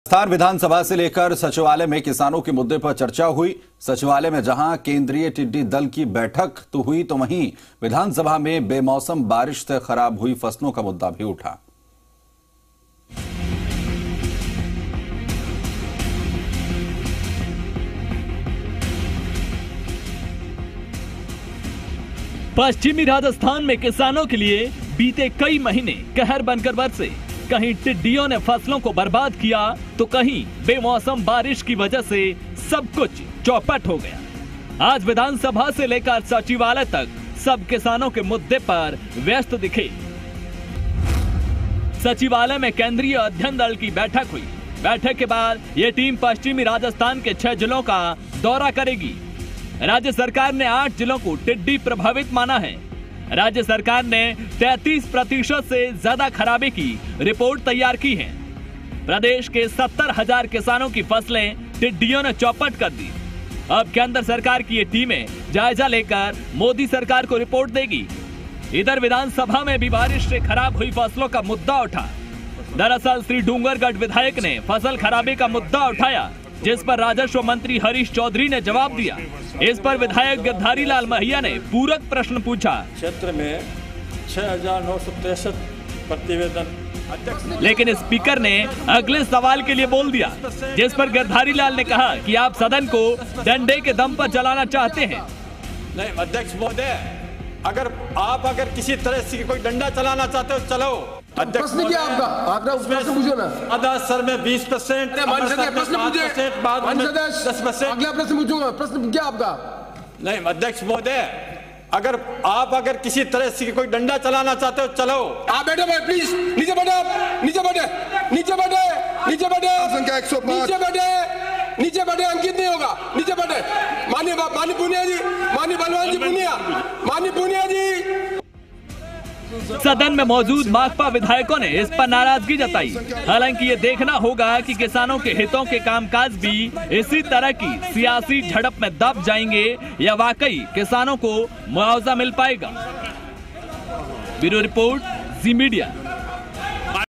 Jaipur : सचिवालय में केंद्रीय दल की बैठक, टिडि्डयों के हमले पर हुई चर्चा
जयपुर : सचिवालय में केंद्रीय दल की बैठक, टिडि्डयों के हमले पर हुई चर्चा